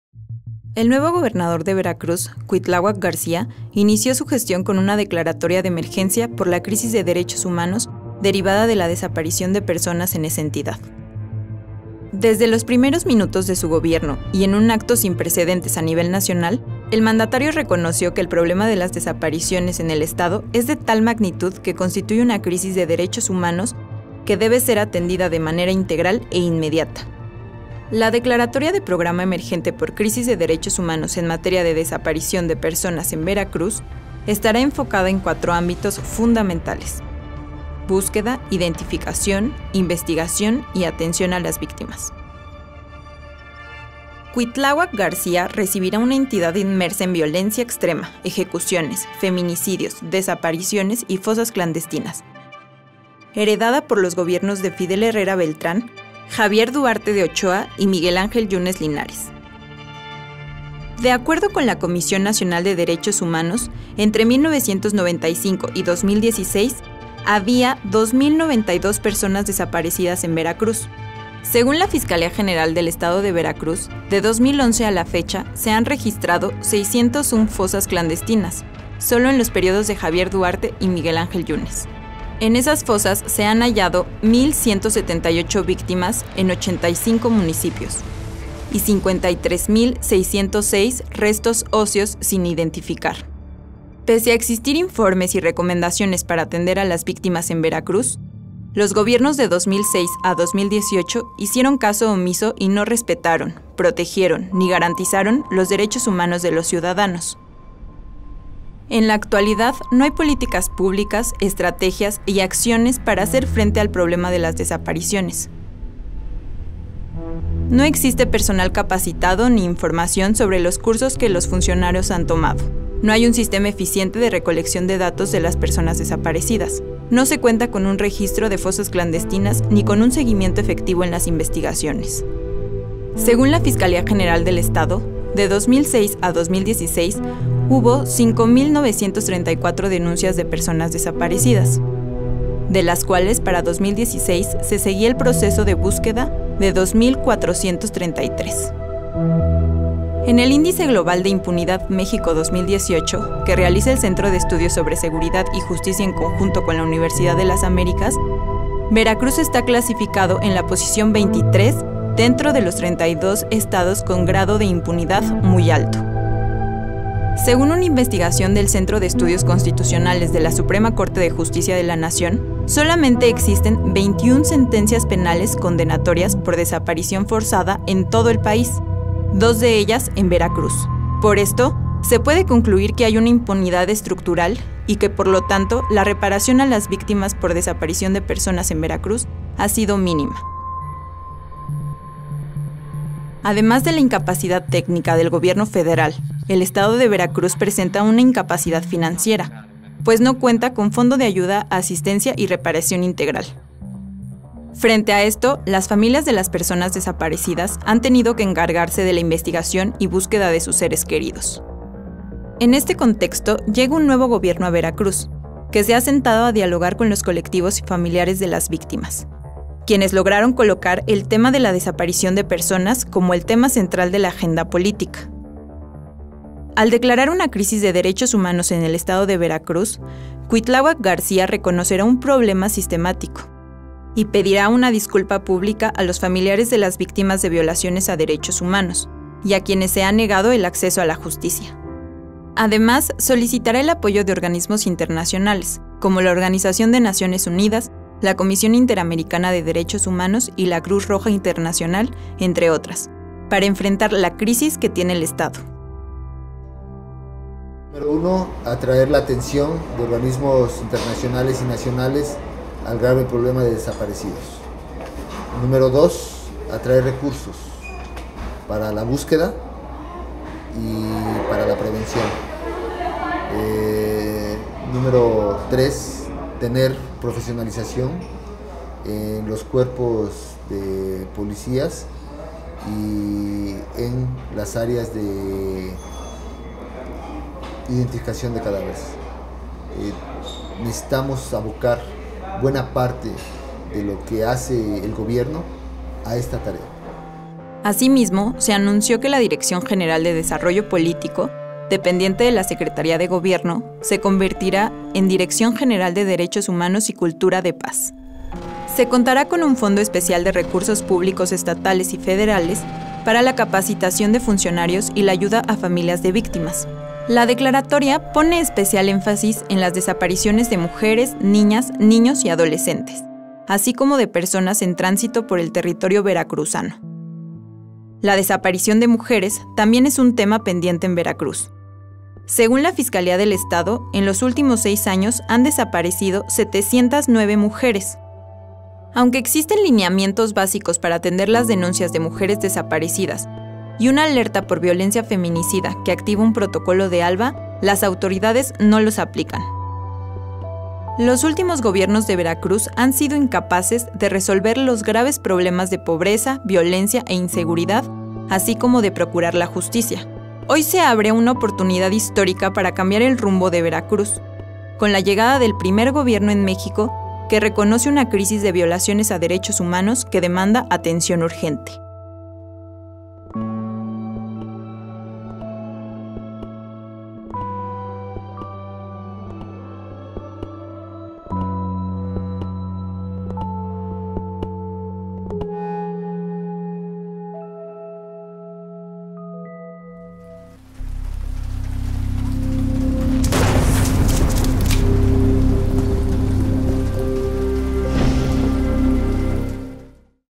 Gobernador Cuitláhuac García - Veracruz, primer estado en decretar crisis de derechos humanos
En un acto sin precedentes en México, el nuevo gobernador del estado de Veracruz, Cuitláhuac García, inició su gestión con una declaratoria de emergencia por la crisis de derechos humanos derivada de la desaparición de personas en esa entidad. Con esto se convierte en el primer gobierno en México que reconoce una crisis humanitaria.